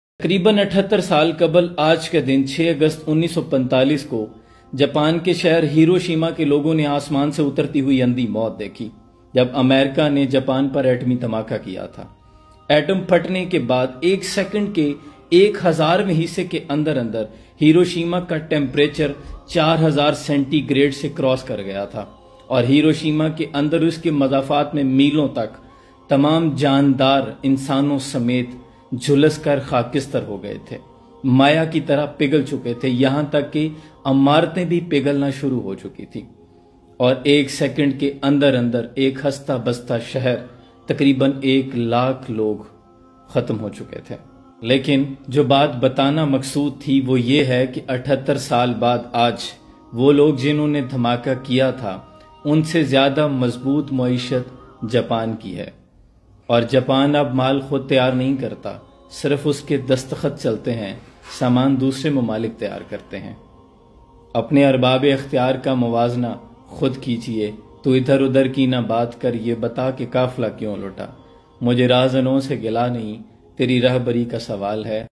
6 August 1945: USA Drops Atomic Bomb On Hiroshima || The Day In History
Dear Viwers !